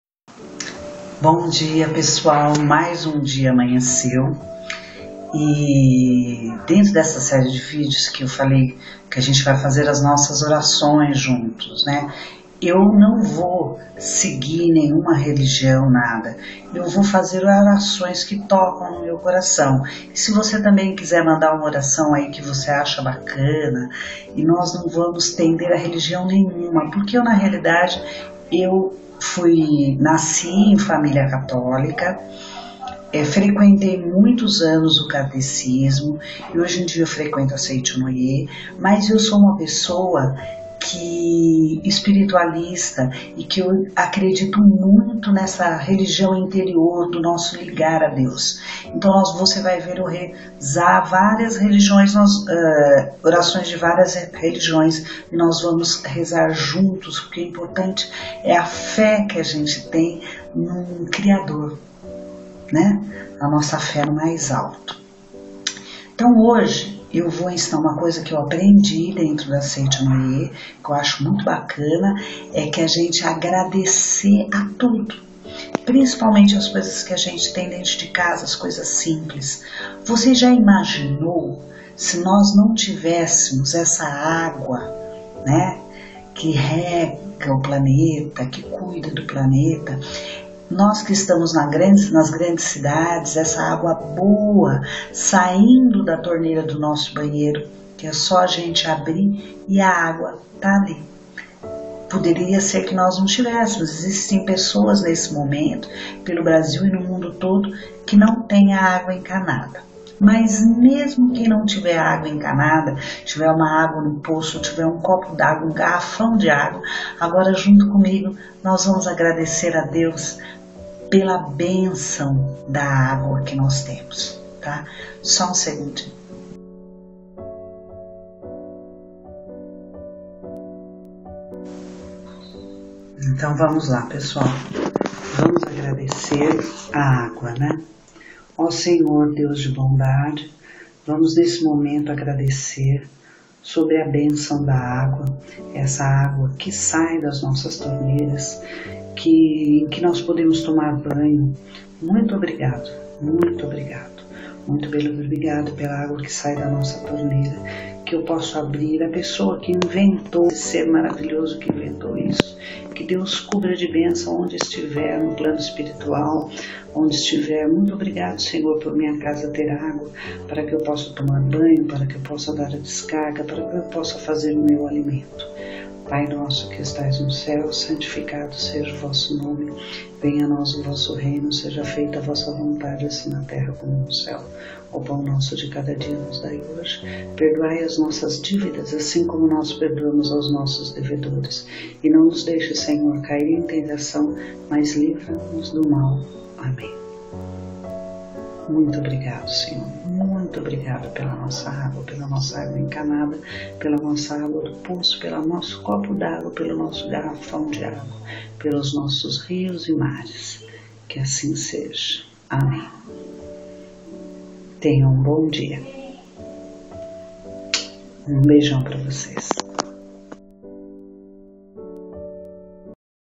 vamos rezar juntos 1
vamor rezar mais uma vez juntos; agradecendo as bençãos do dia a dia!
fatima borges silva